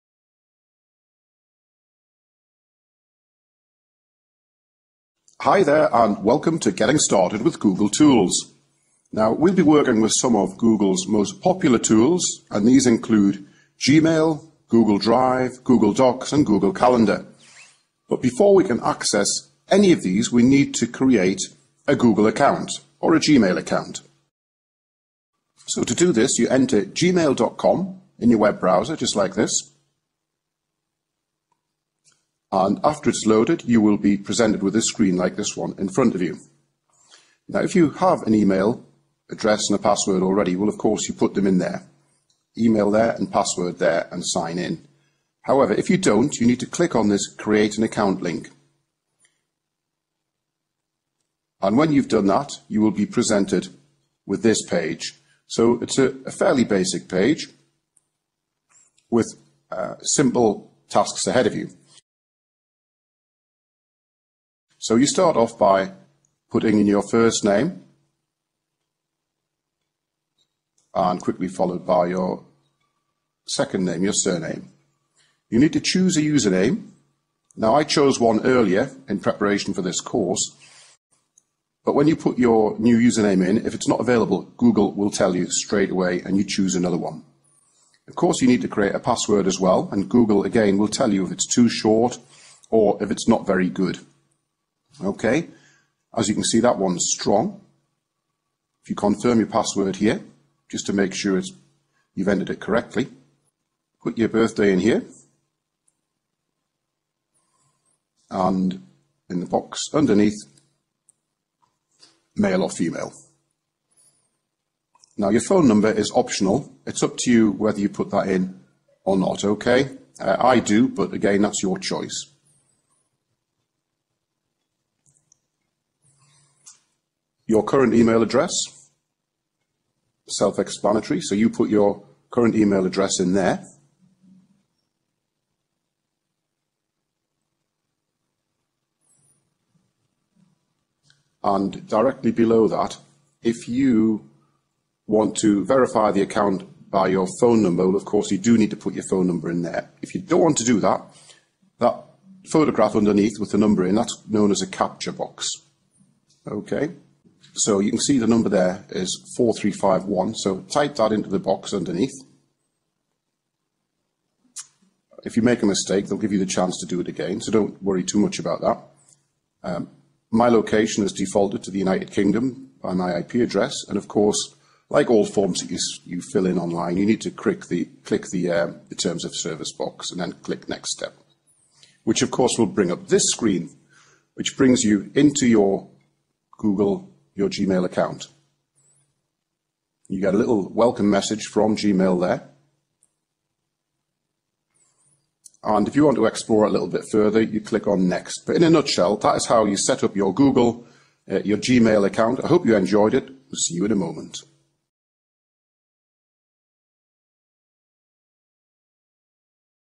How to create a Gmail account : How to create Google account 2017 : How to CREATE Gmail ID
in this video you will learn about how to create a google gmail account with new method with online classes